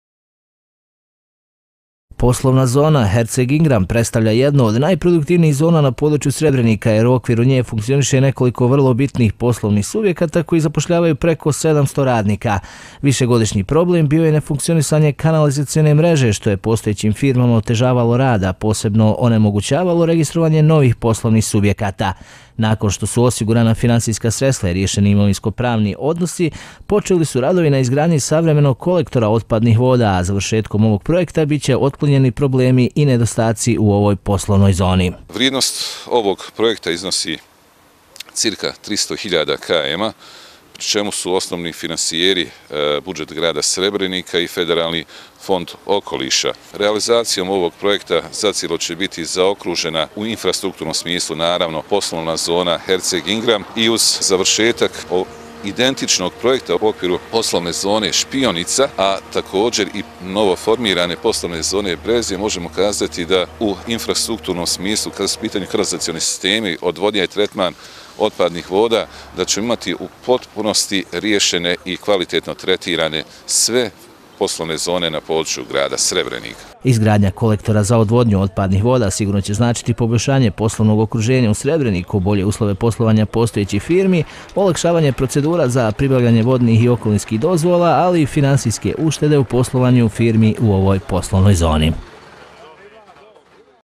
SREBRENIK RADOVI NA KANALIZAC KOLEKTORU 22 04 2021
U poslovnoj zoni Herceg-Ingram u Srebreniku trenutno se nalazi nekoliko veoma značajnih firmi proizvođača i izvoznika, a sve zajedno zapošljavaju više od 700 radnika. Ono što je već duže vrijeme predstavljalo problem i nedostatak ove poslovne zone, jeste nedostatak kvalitetne kanalizacione mreže, no ovih dana počelo je rješavanje ovog problema i to izgradnjom savremenog kolektora otpadnih voda.